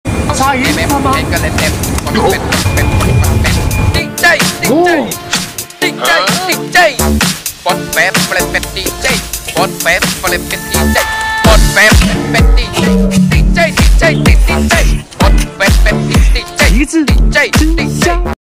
Does anyone know what kind of fish head this is?ASMR Tik Tok#FYP
I firmly believe in respecting and protecting animals, and all the ingredients in the video are obtained in compliance with relevant ethical and legal guidelines. Does anyone know what kind of fish head this is?ASMR Tik Tok#FYP